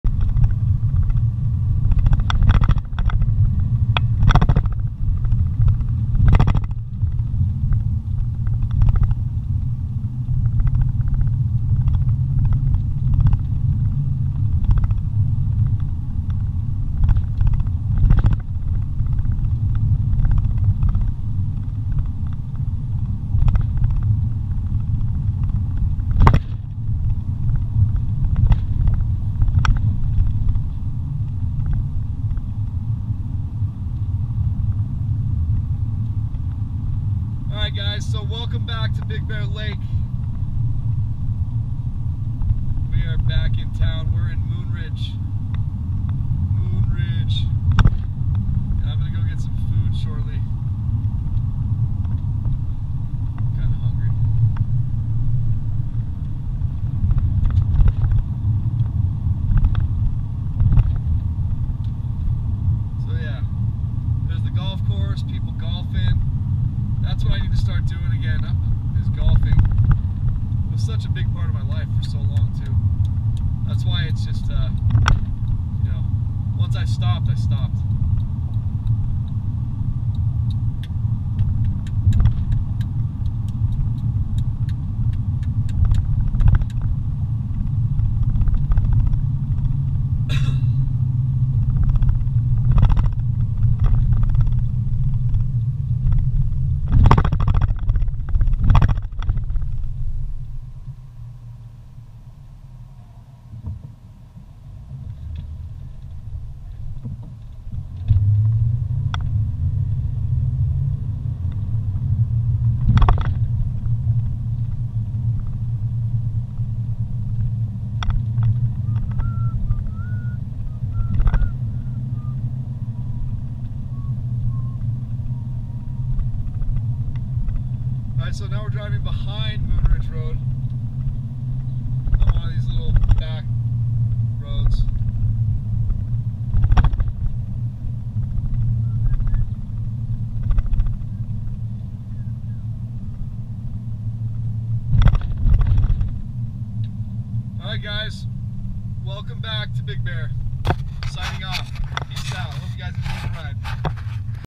Taking 2N10 off-roading in the San Bernardino Mountains behind Snow Summit. 5/26/17. Part 5 of 5
Probably the most beautiful drive ive ever been on. I forgot how gorgeous this road is. And especially early AM.

2N10 is the prettiest off-road in Big Bear area.